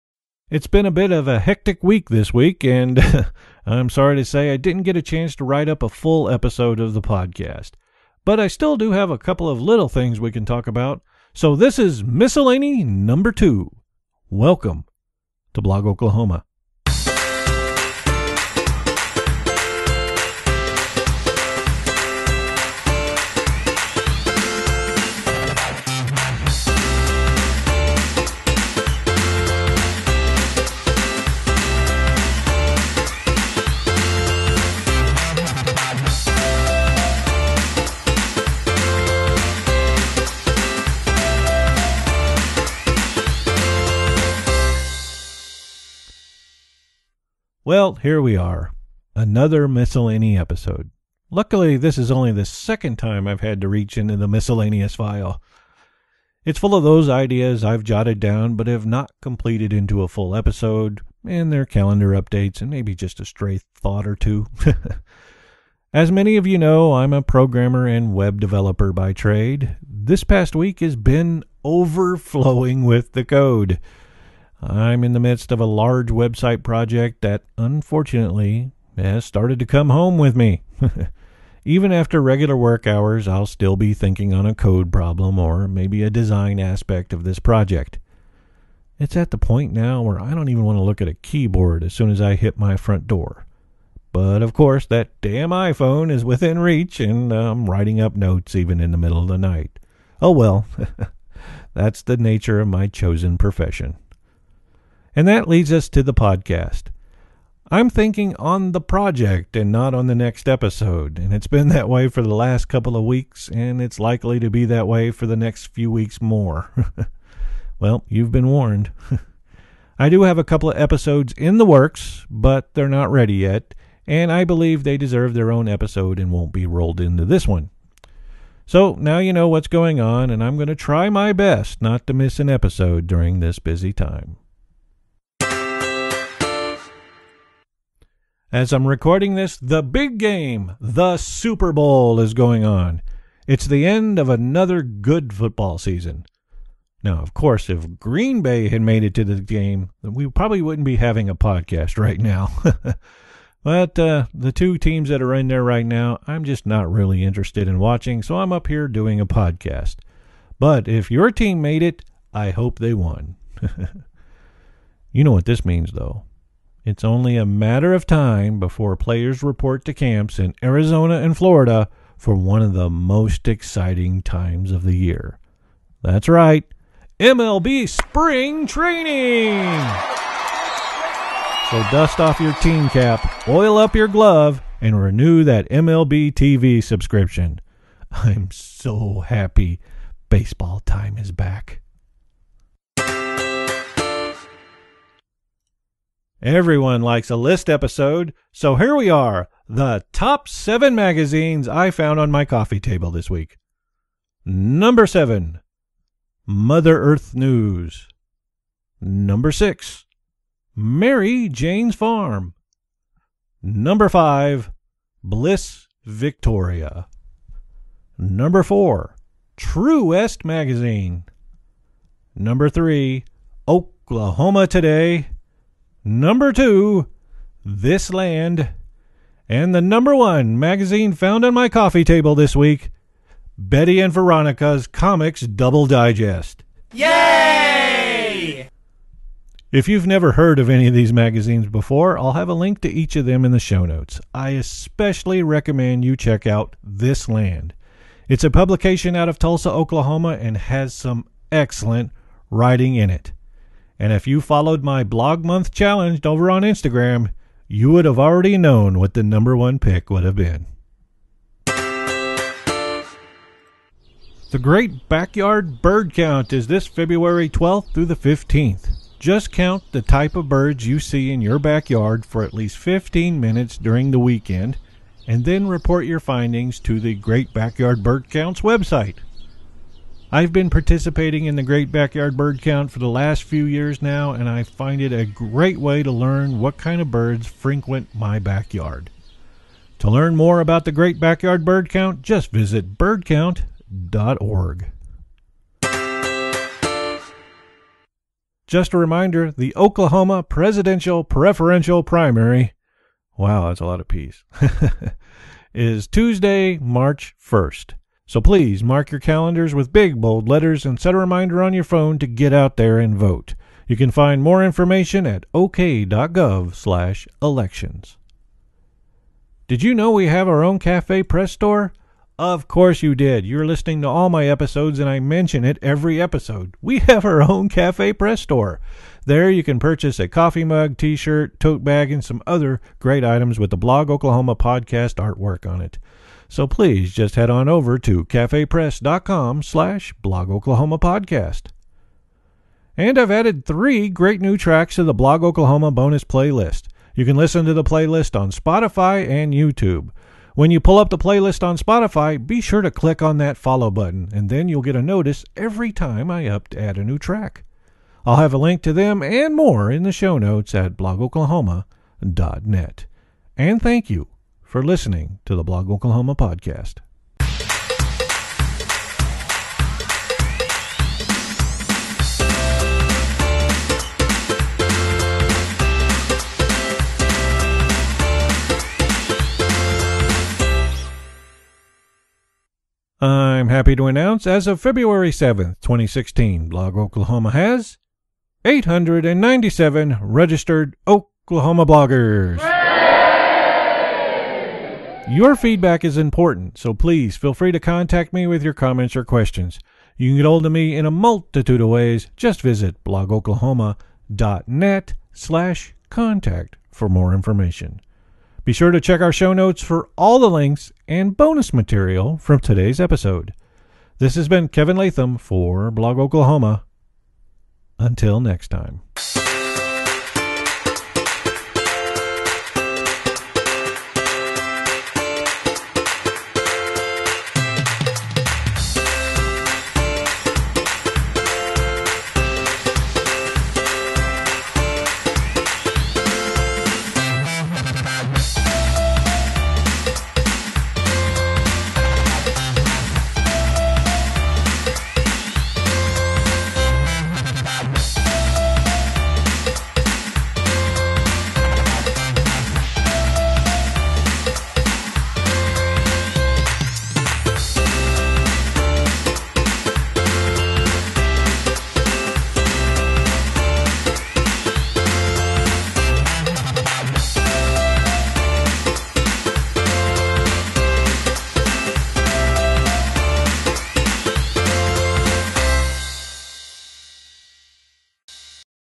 Blog Oklahoma Podcast 023: Miscellany 2
[audio] February 7, 2016 -  It’s been a bit of a hectic week, and I’m sorry to say didn’t

get a chance to write up a full episode. But I do have a few little things we can talk

about. So this is Miscellany 2. Welcome to Blog Oklahoma.

Just a reminder the Oklahoma Presidential Preferential Primary is Tuesday, March 1st. The

deadline to request an absentee ballot is by 5 p.m. on February 24th. So please mark your

calendars with big bold letters and set a reminder on your phone to get out there and vote.

You can find more information at ok.gov/elections.

Are you someone who blogs in or about Oklahoma? Then you already qualify for web ring

membership. Join Blog Oklahoma today!